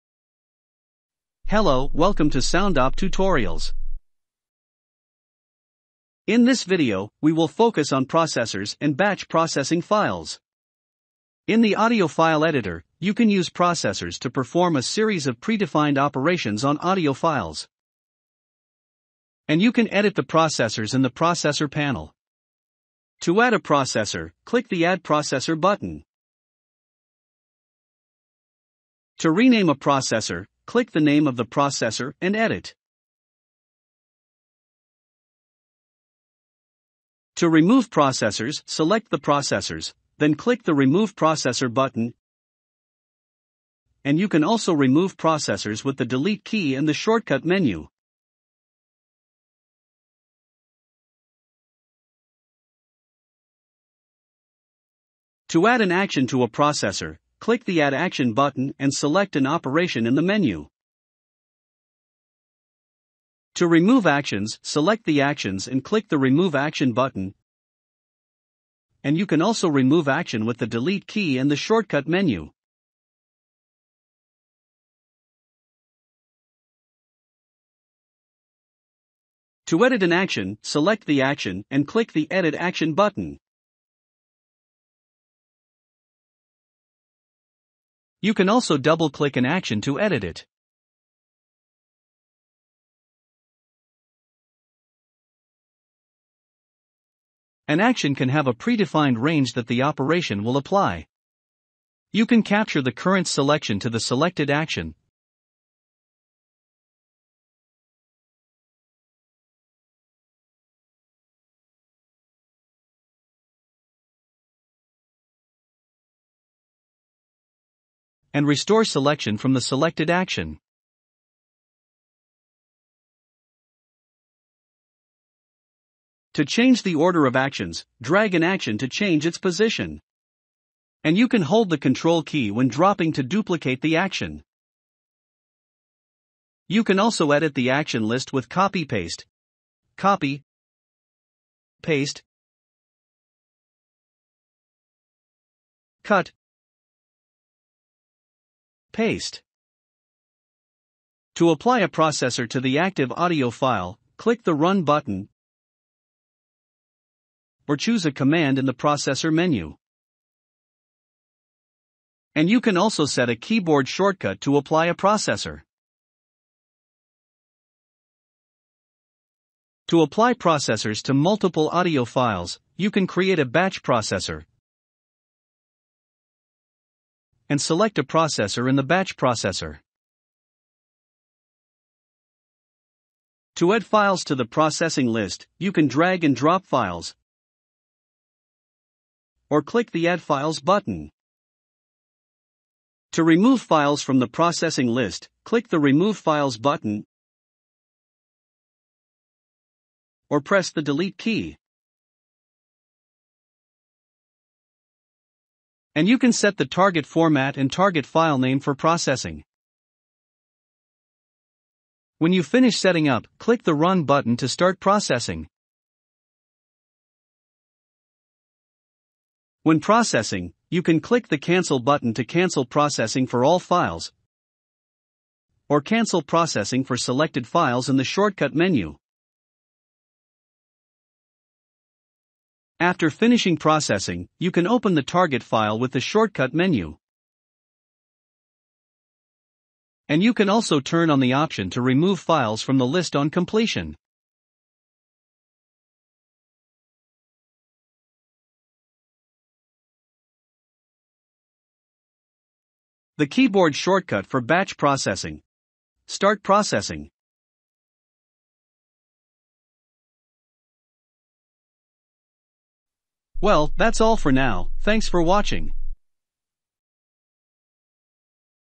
Processors and Batch Processing | Soundop Tutorials
This video demonstrates how to use processors in the audio file editor and how to batch process audio files with the batch processor.